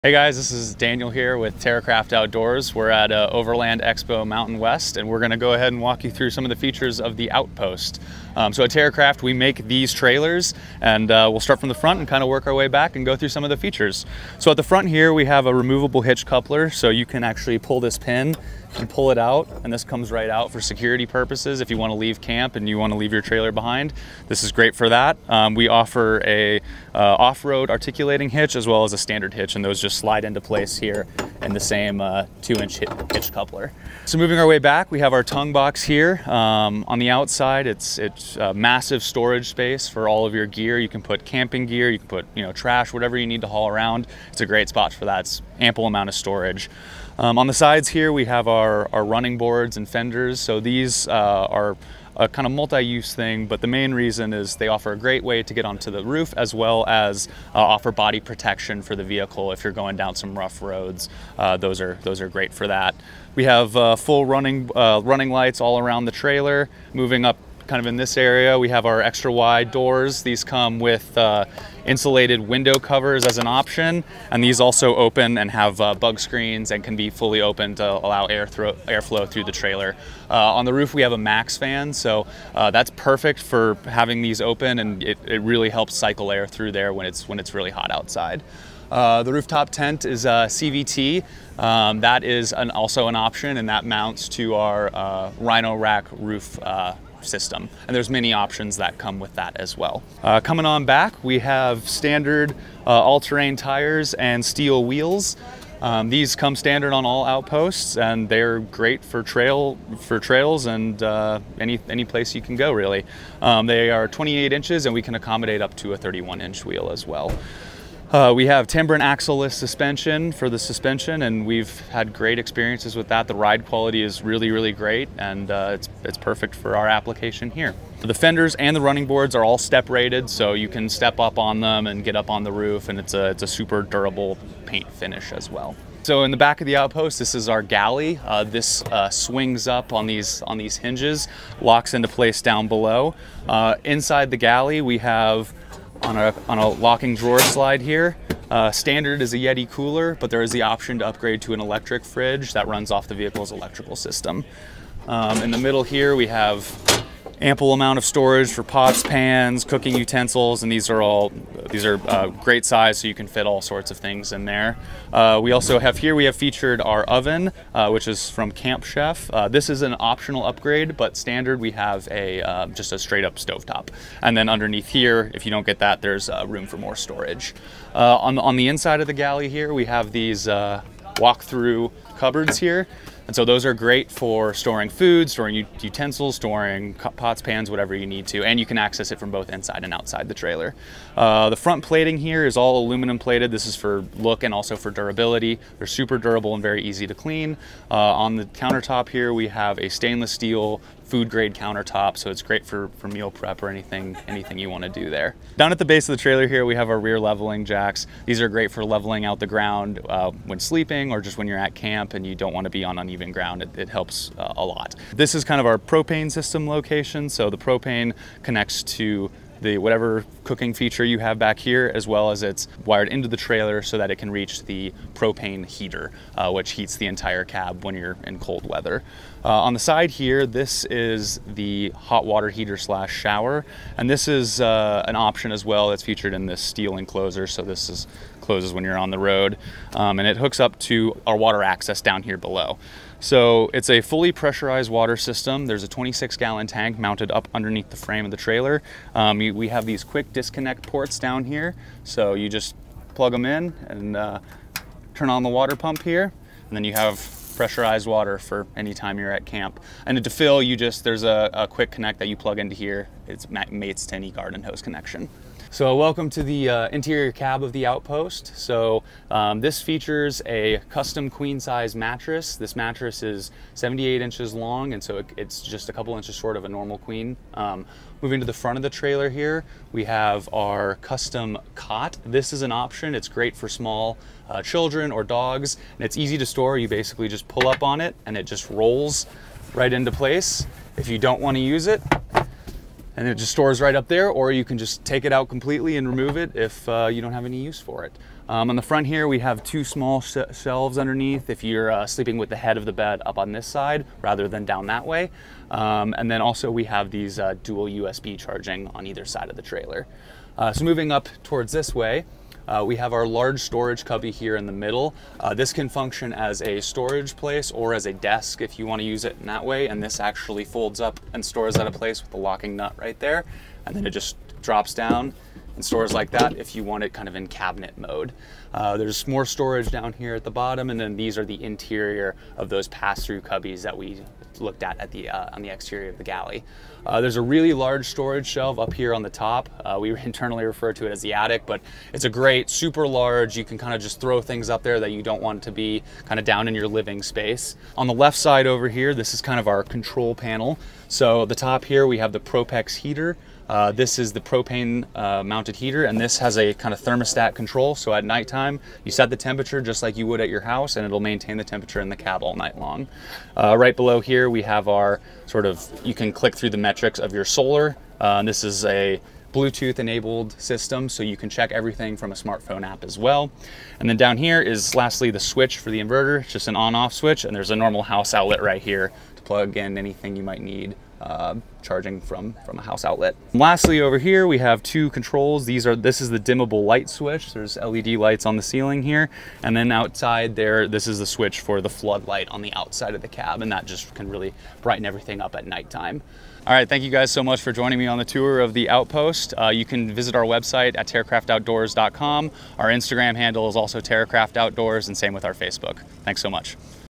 Off Road Teardrop Camper Trailer by Terracraft Outdoors
This video features Off Road Teardrop Camper Trailer by Terracraft Outdoors at @OverlandExpo  Mountain West 2021 in Loveland, CO.

Purchases made through some store links may provide some compensation to @WelcomeToTheOutdoors